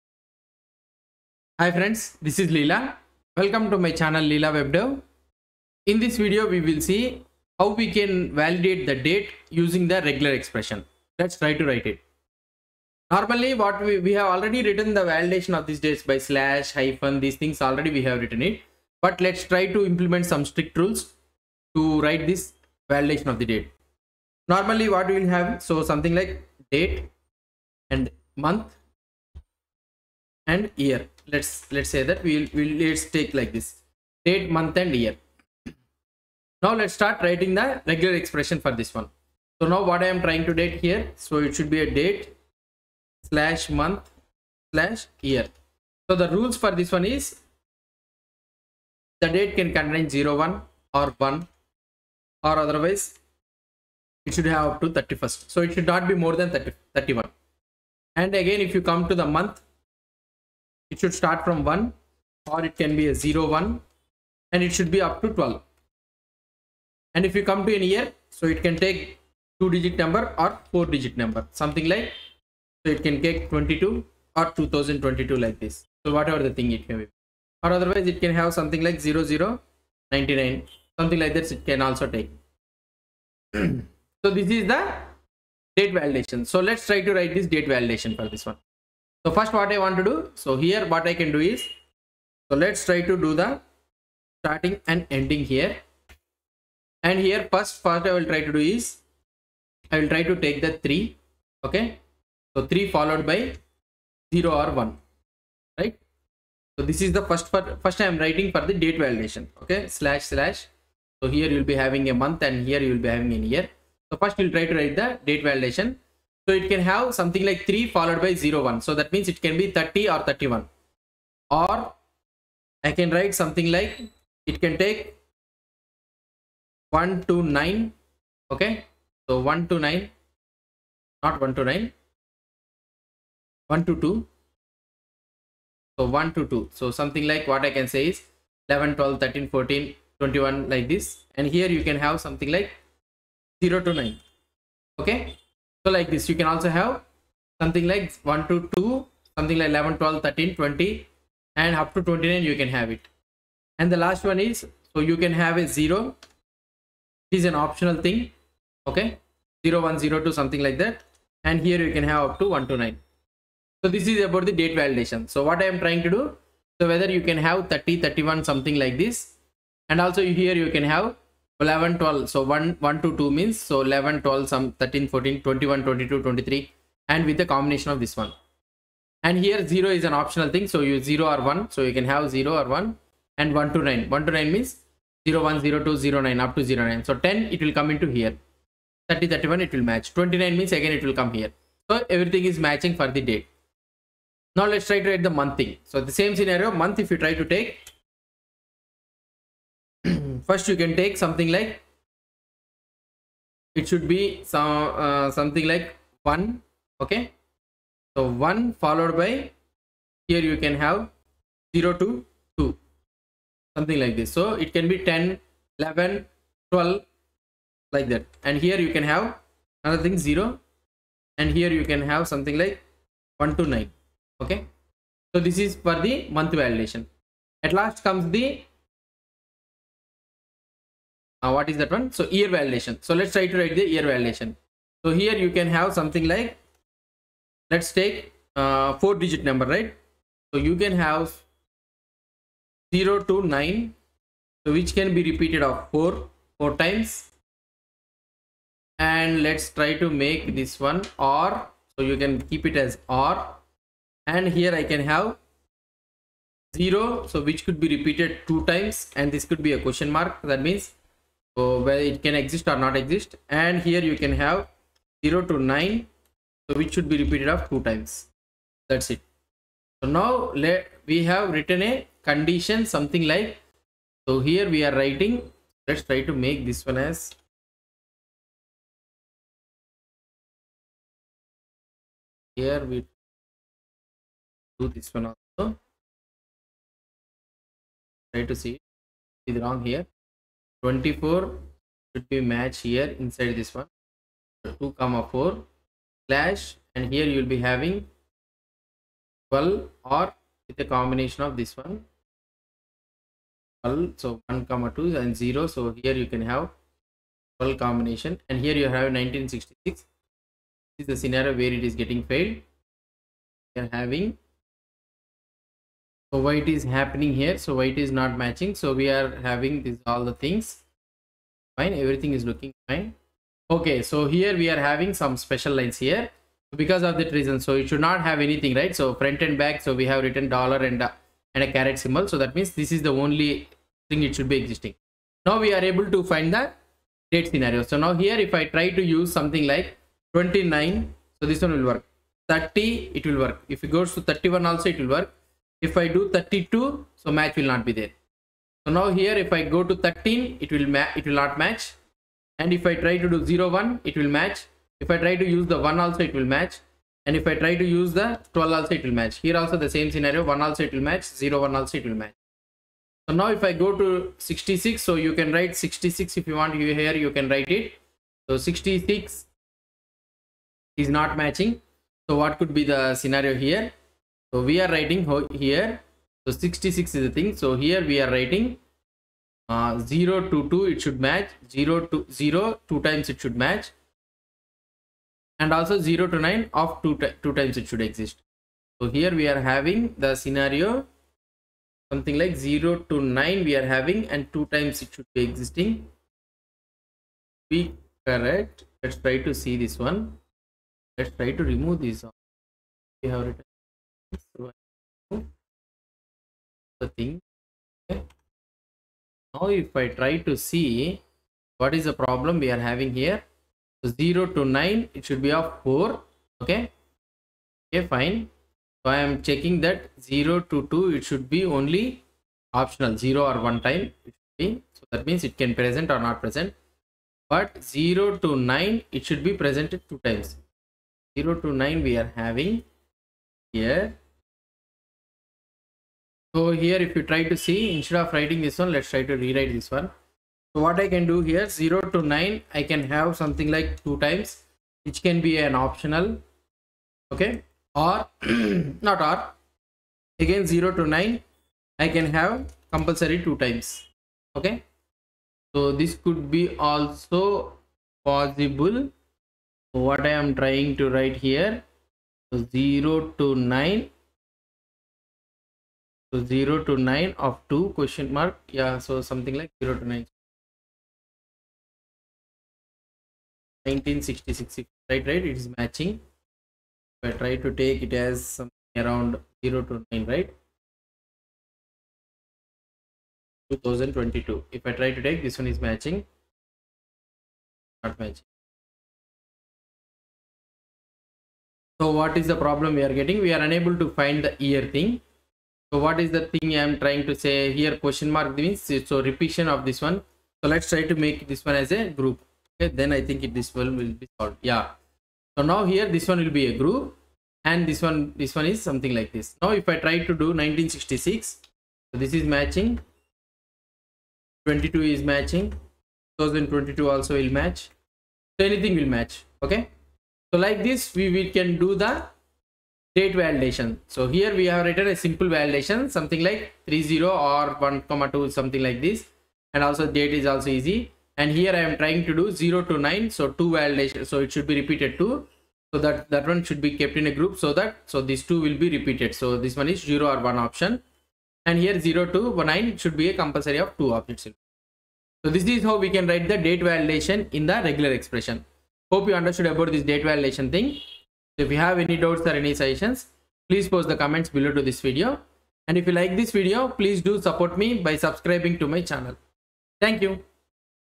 34. Validating the Date Format using Regular Expression - RegEx
In this video we will see how to validate the date format using Regular Expression - RegEx